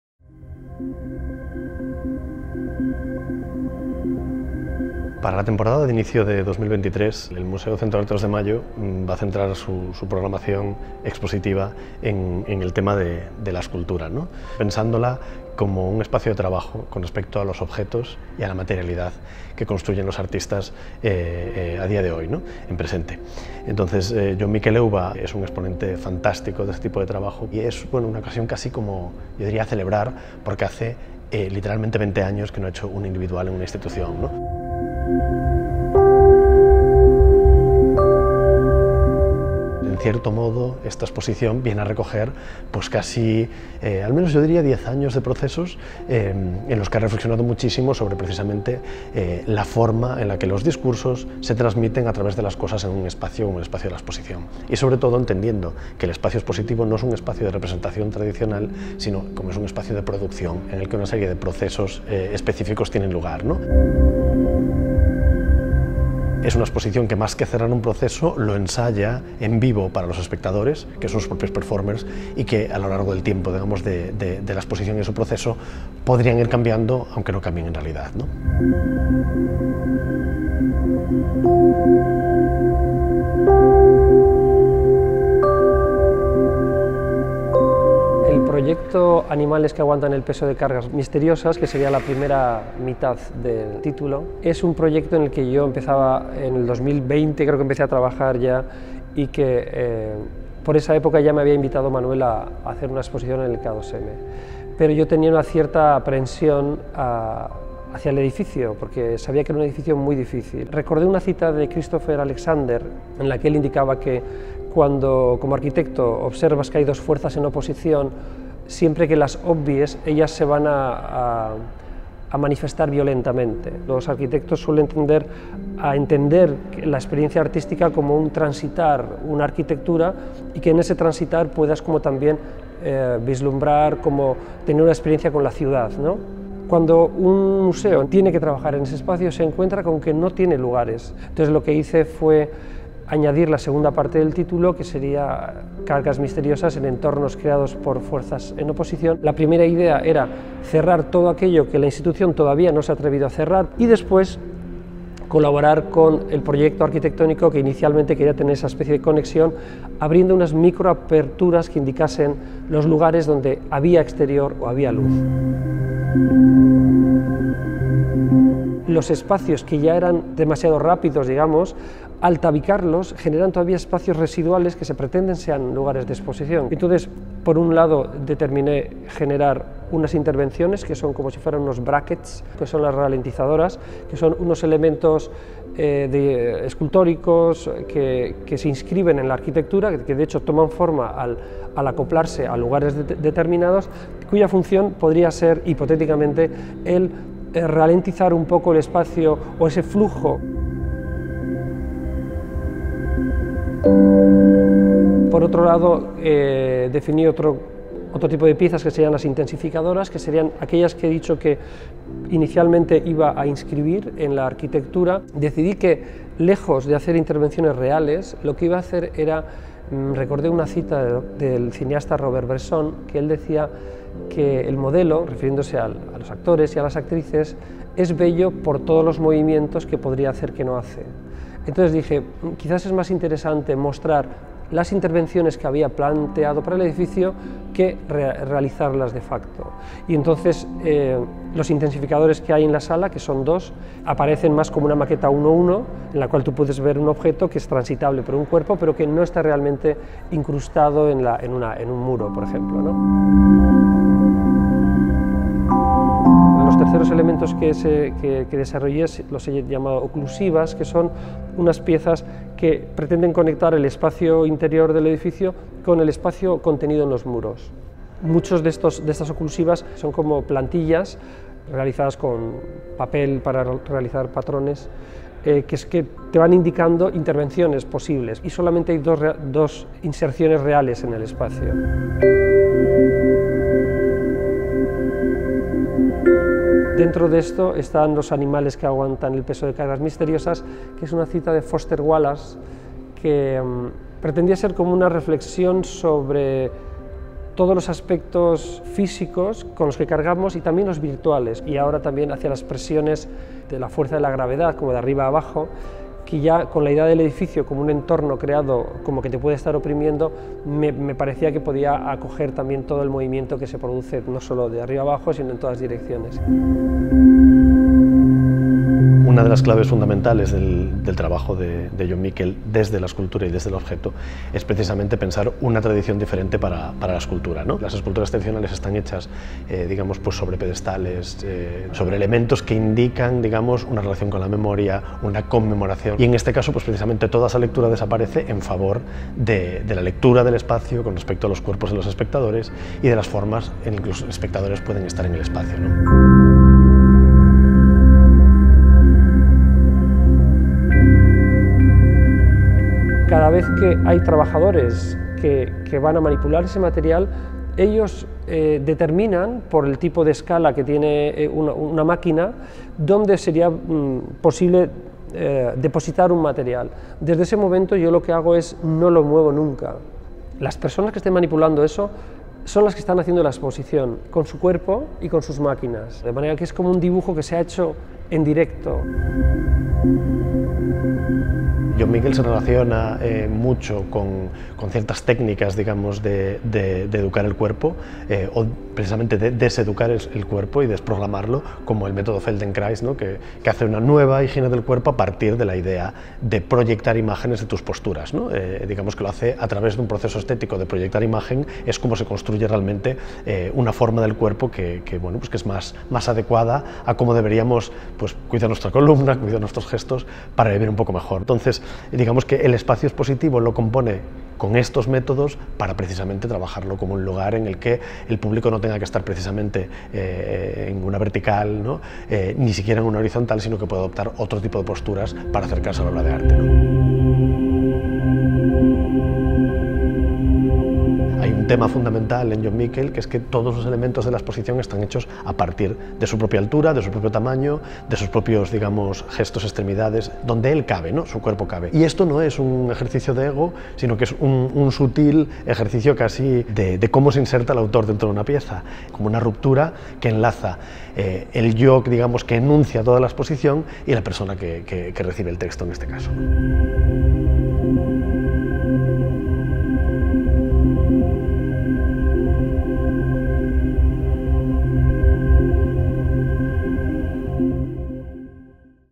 JON MIKEL EUBA. 19 enero - 23 abril 2023.
"Animales que aguantan el peso de cargas misteriosas en entornos creados por fuerzas en oposición".

Primera exposición individual en una institución desde el año 2003, para Jon Mikel Euba, la fascinación conceptual es la de "el hacer". Esta metodología implica trabajo colectivo, negociación y discusión permanentes que se ofrecen al espectador en una serie de gestos en el cuerpo del edificio desplazando la noción de escultura tradicional.

Comisariado por Manuel Segade.

Etiquétanos